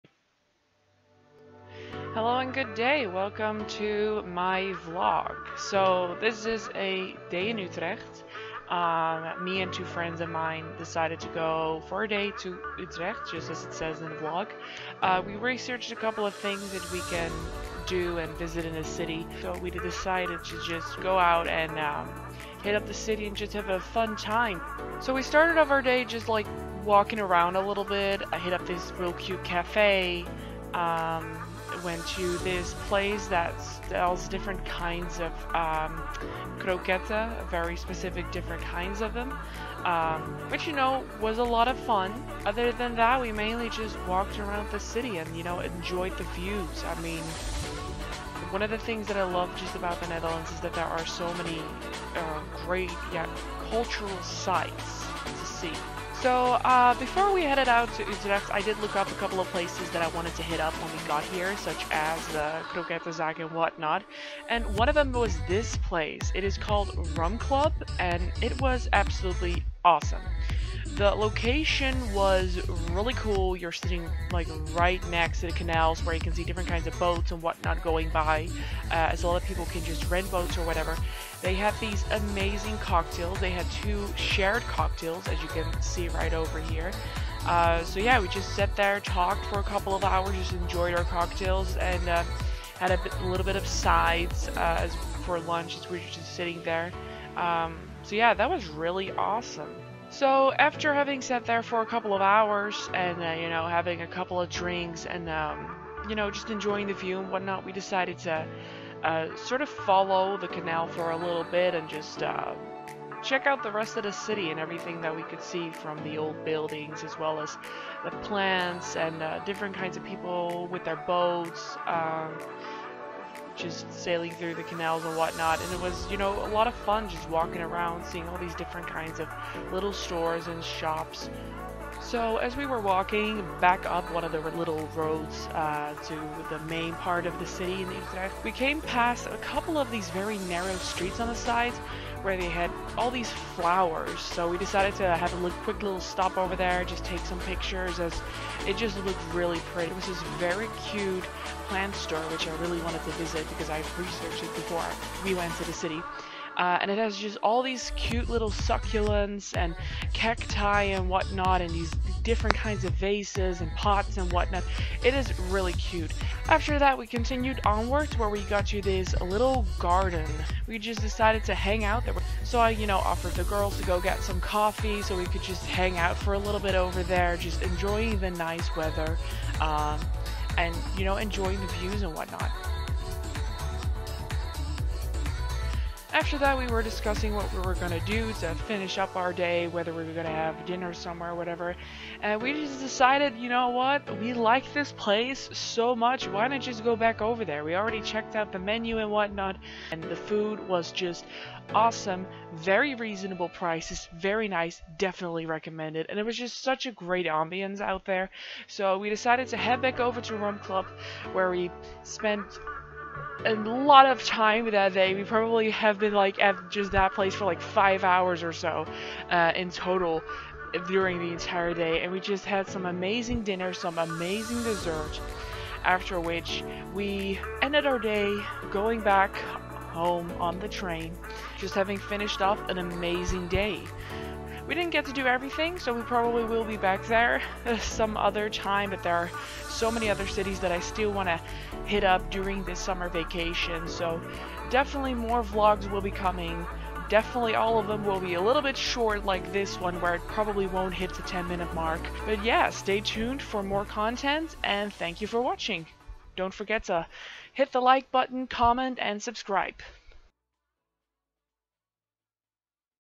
A day in Utrecht [Vlog in the Netherlands] June 2020
Hi guys!

In this short vlog I'll be showing you bits and pieces of my last day trip to Utrecht. As summer vacation is just coming to a start you can count on more vlogs yet to come.

Places visited:
Rum Club